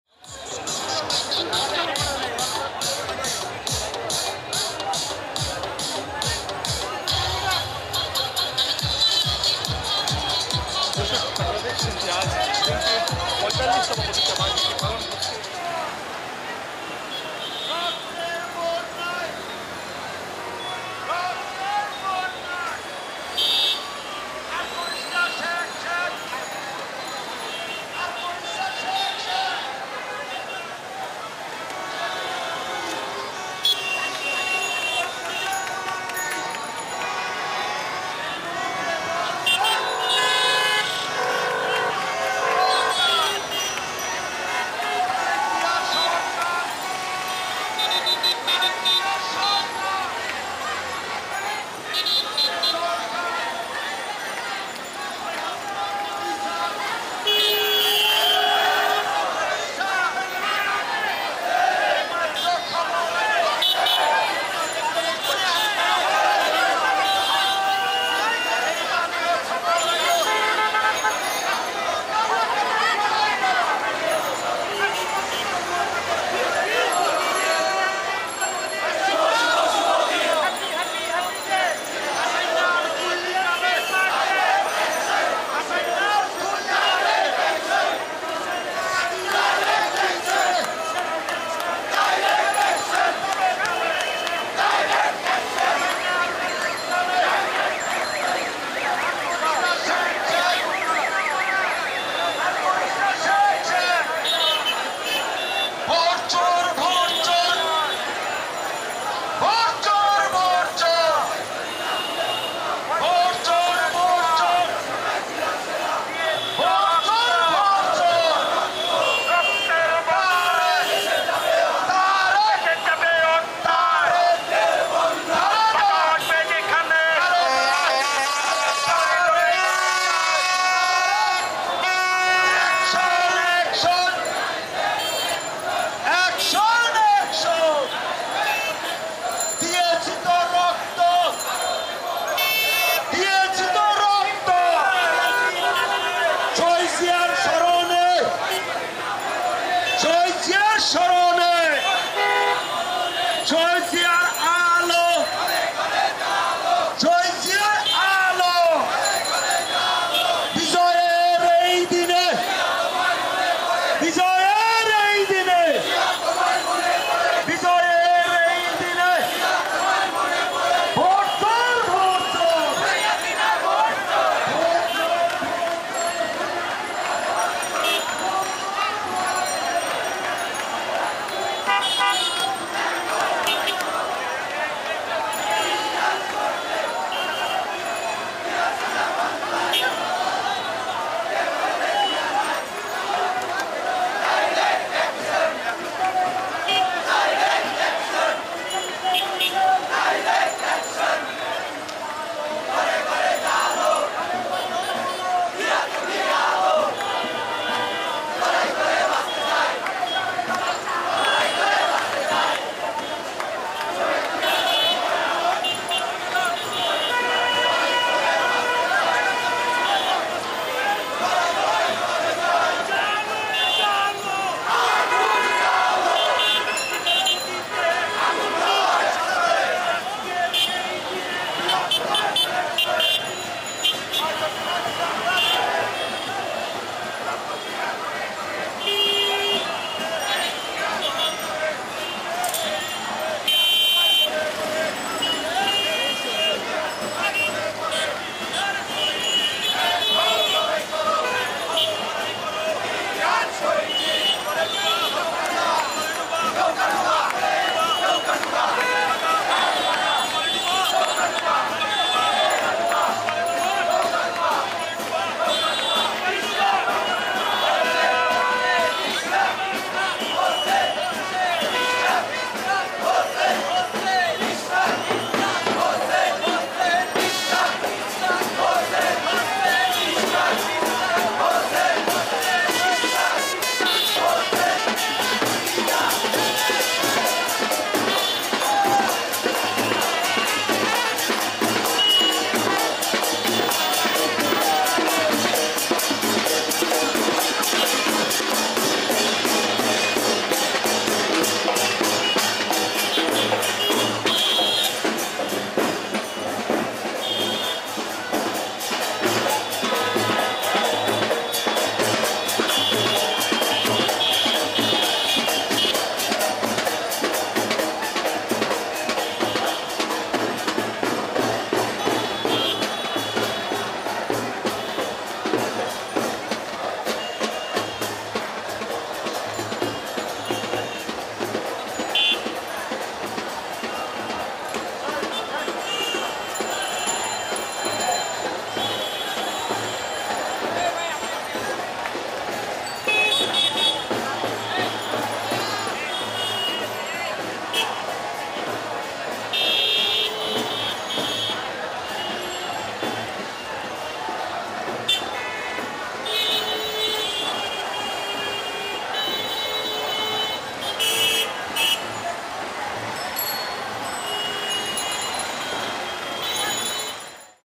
বিএনপি'র ৪৫তম প্রতিষ্ঠাবার্ষিকী উপলক্ষে আয়োজিত শোভাযাত্রা
বিএনপি'র ৪৫তম প্রতিষ্ঠাবার্ষিকী উপলক্ষে আয়োজিত শোভাযাত্রায় পুরান ঢাকা থেকে মতিঝিল হয়ে নয়াপল্টনে যোগ দেই।

তারিখ: ০১/০৯/২০২৩